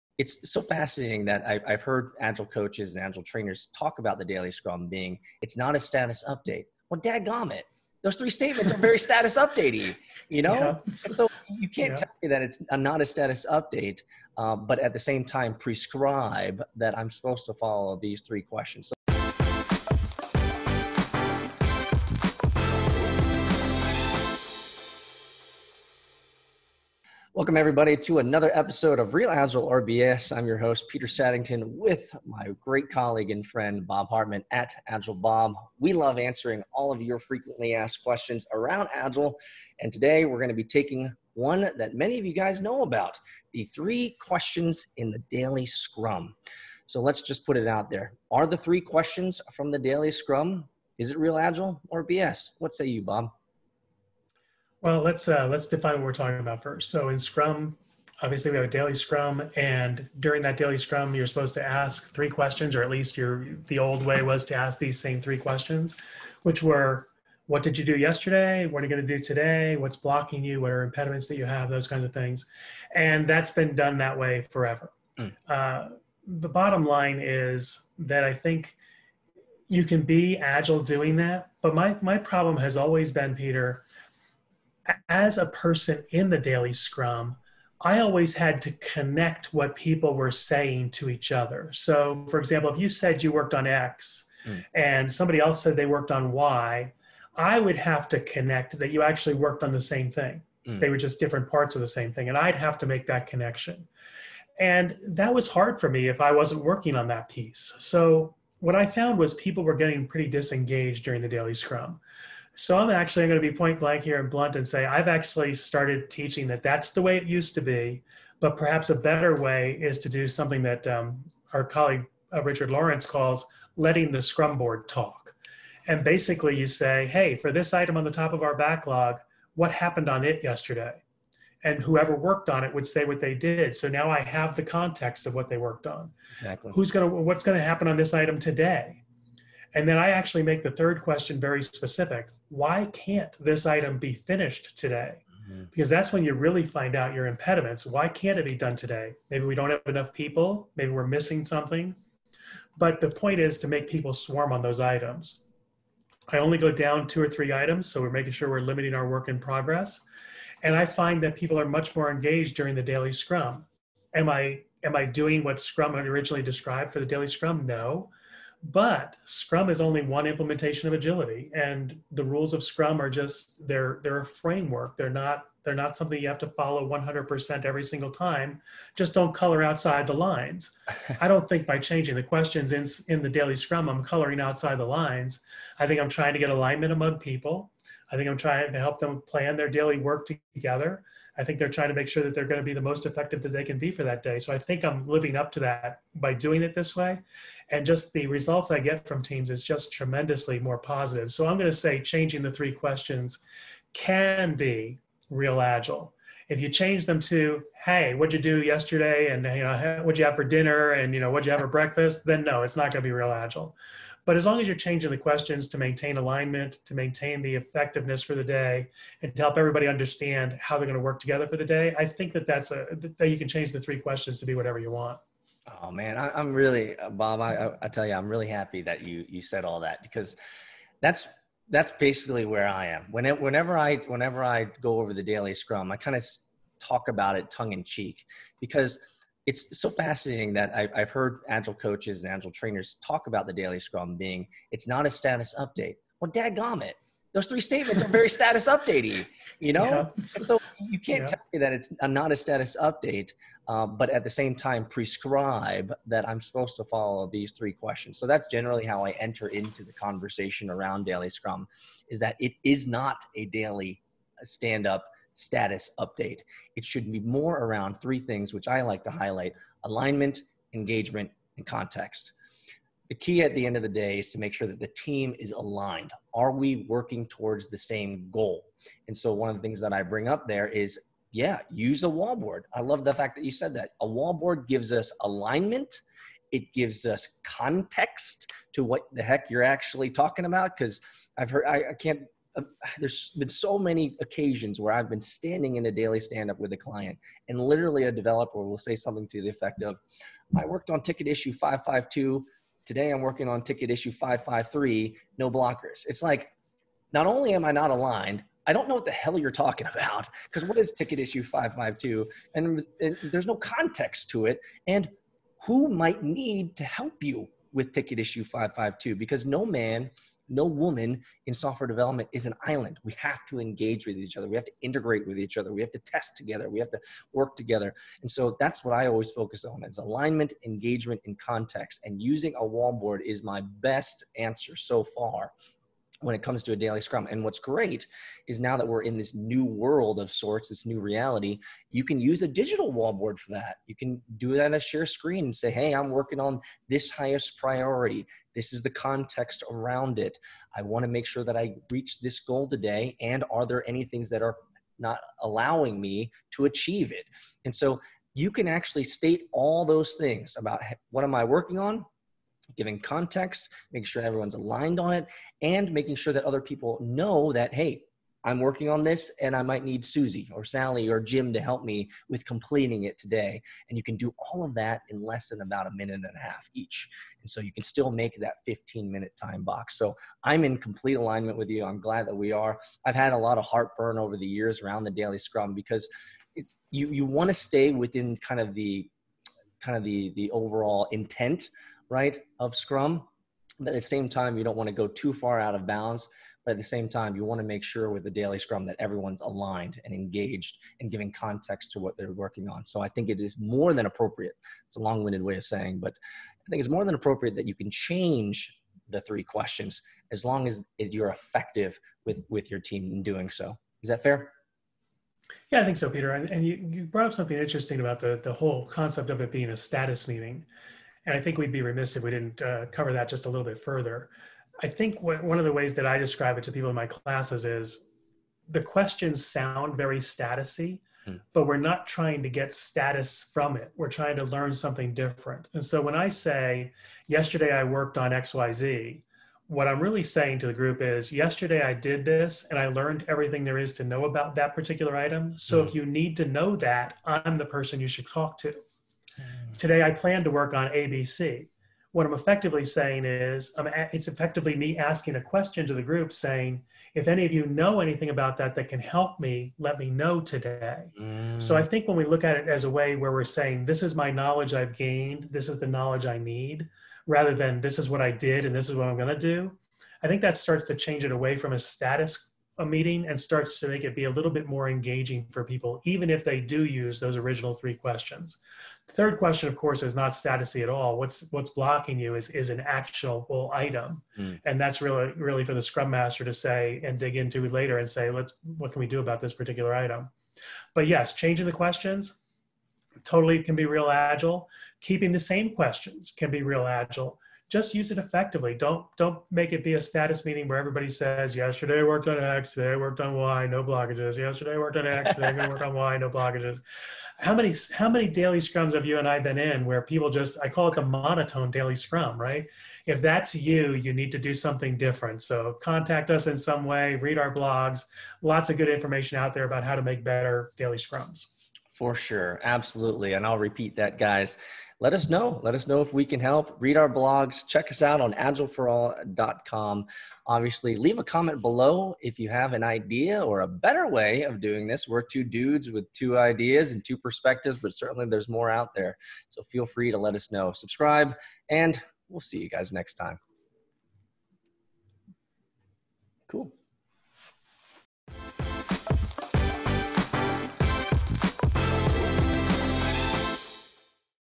[Real Agile or BS?] Not Using the 3 Questions in the Daily Scrum? Real Agile or BS?
Are you using the 3 questions in the Daily Scrum? Agile Bob (Bob Hartman) and Agile Peter (Peter Saddington) weigh in on how they are teaching the Daily Scrum and whether they think the 3 questions are outdated. Is it Real Agile... or BS? You tell us!

Level up with Bob Hartman or Peter Saddington in one of their upcoming courses